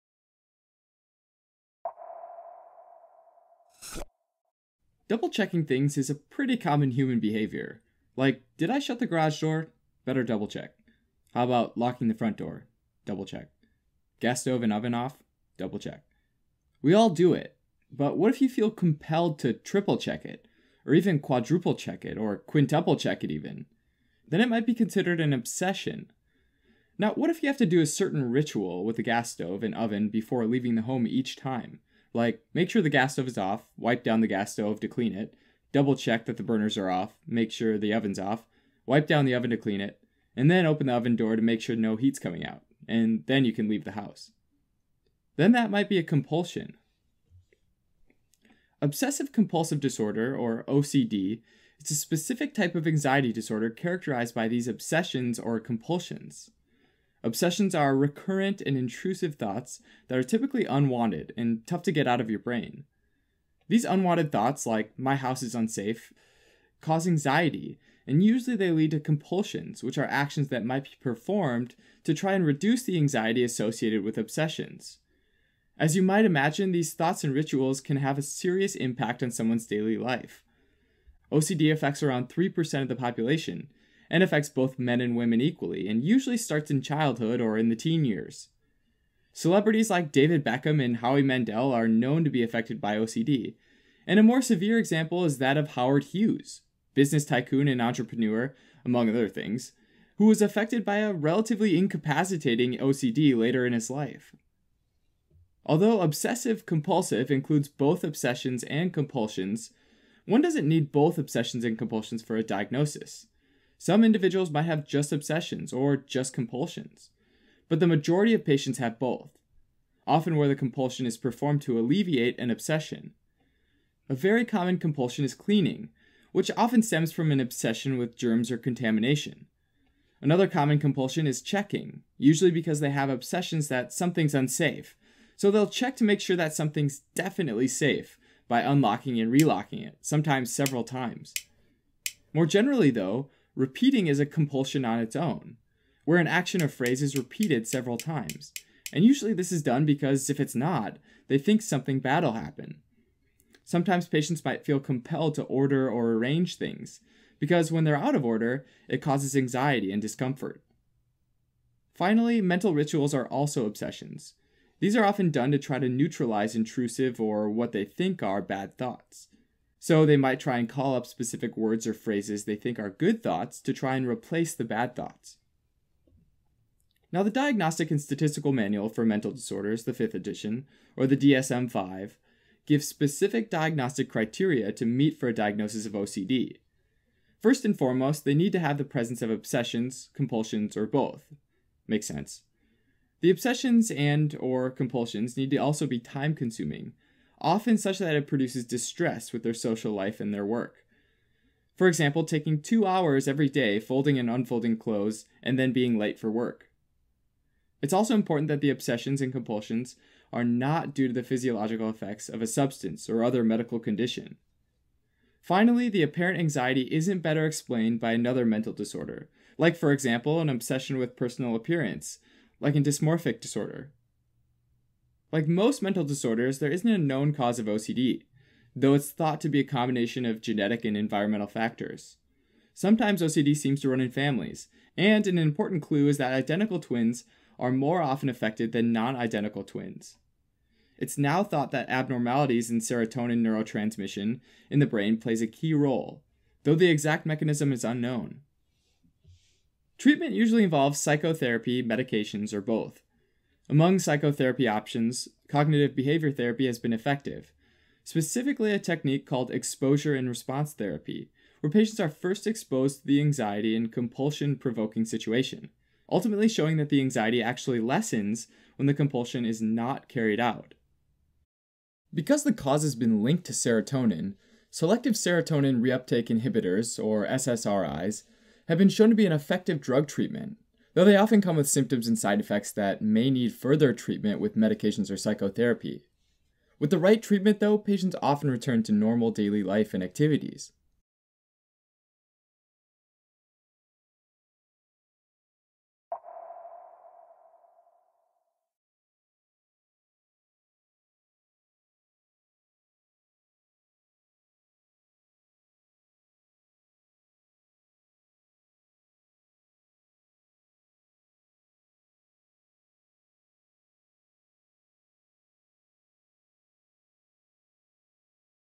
Obsessive compulsive disorder (OCD) - causes, symptoms & pathology
What is obsessive-compulsive disorder (OCD)? OCD's a condition where somebody has intrusive and anxiety-inducing thoughts called obsessions, that often compel them to carry out actions in order to reduce the anxiety—called compulsions. Find our full video library only on Osmosis